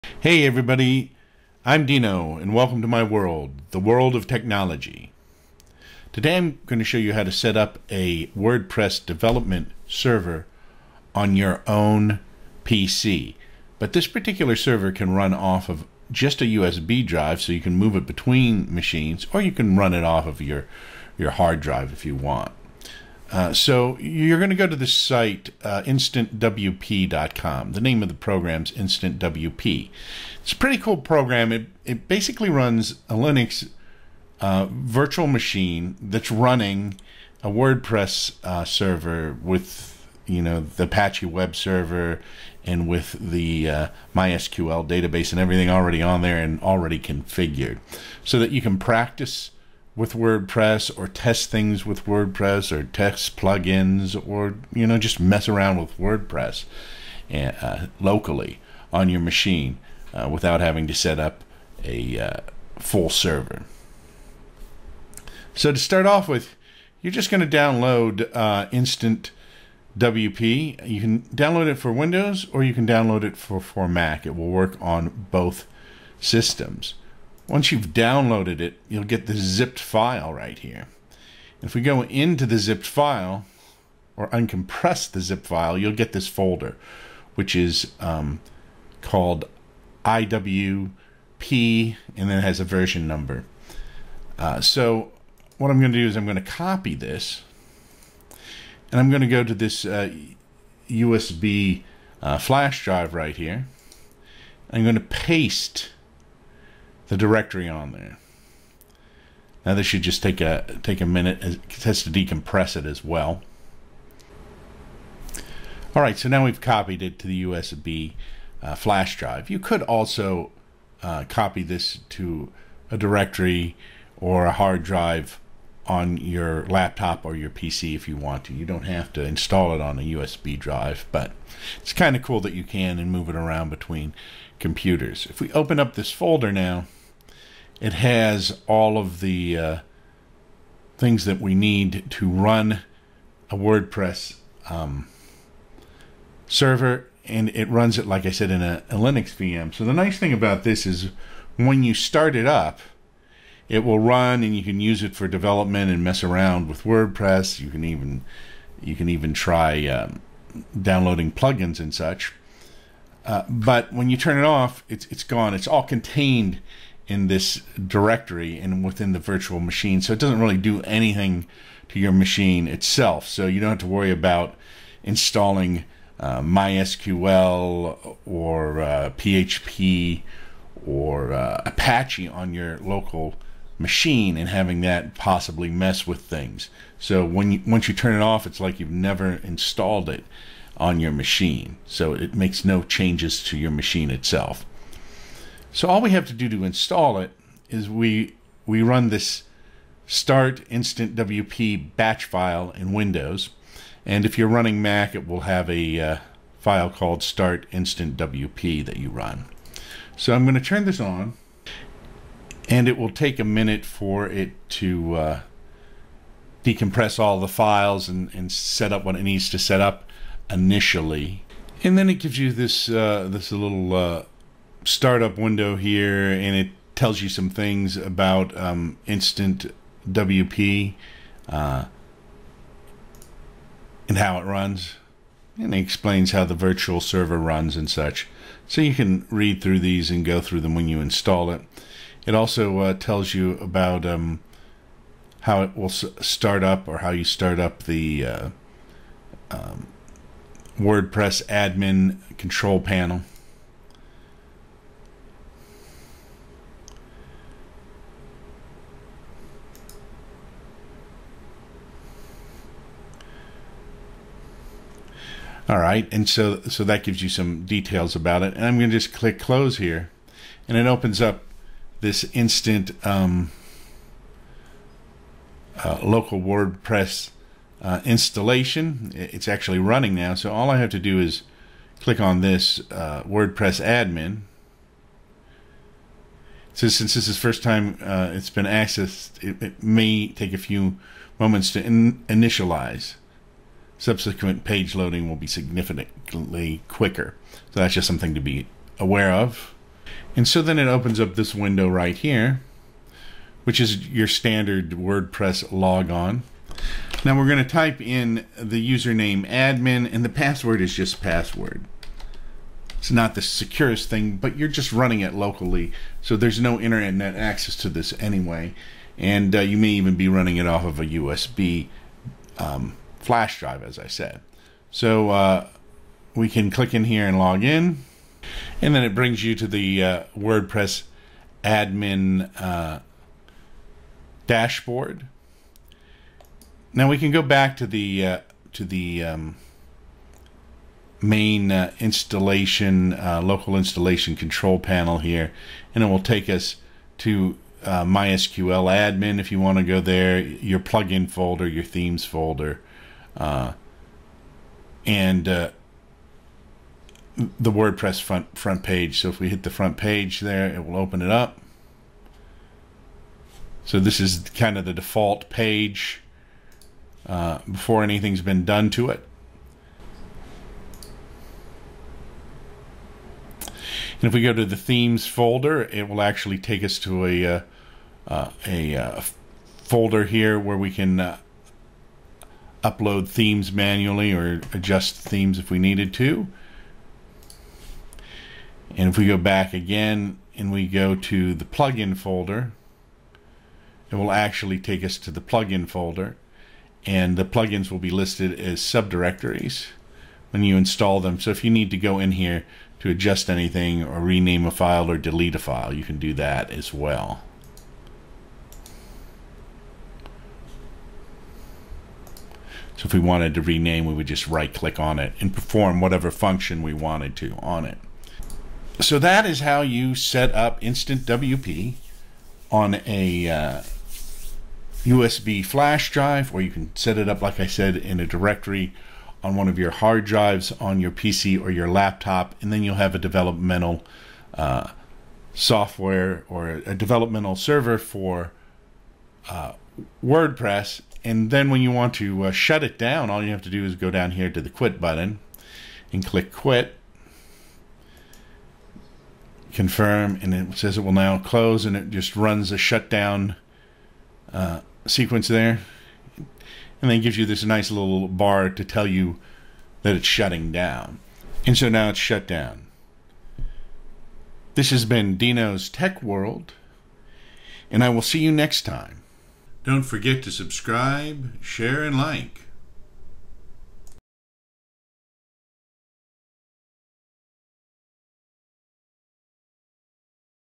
InstantWP - How to Install a Local WordPress Development Server on a USB Flash Drive or Hard Drive
How to Set Up a WordPress Development Server on Your PC or USB Drive | Dino's Tech World

Hey everyone, Dino here! In today’s video, I’m showing you how to set up your own WordPress development server using InstantWP. Whether you want to test themes, plugins, or experiment with WordPress, InstantWP lets you run a complete WordPress server on your PC or even directly from a USB drive. No need to install complex server software like Apache or MySQL — it's all built in!

In this tutorial, I’ll guide you through:

Downloading and setting up InstantWP
Running a WordPress server from a USB drive or local PC
Using the WordPress dashboard to manage your site
How to access plugin and theme folders for easy modifications
Safely shutting down your local WordPress server
This is a great solution if you want to work on a local WordPress environment without affecting your actual machine or moving it between different computers.

⏰ Timestamps: 0:00 – Intro and overview of InstantWP 1:00 – Downloading and extracting InstantWP 2:30 – Running WordPress from a USB or local hard drive 4:00 – Accessing WordPress admin and dashboard 5:30 – Managing plugins, themes, and MySQL 7:00 – Shutting down the WordPress server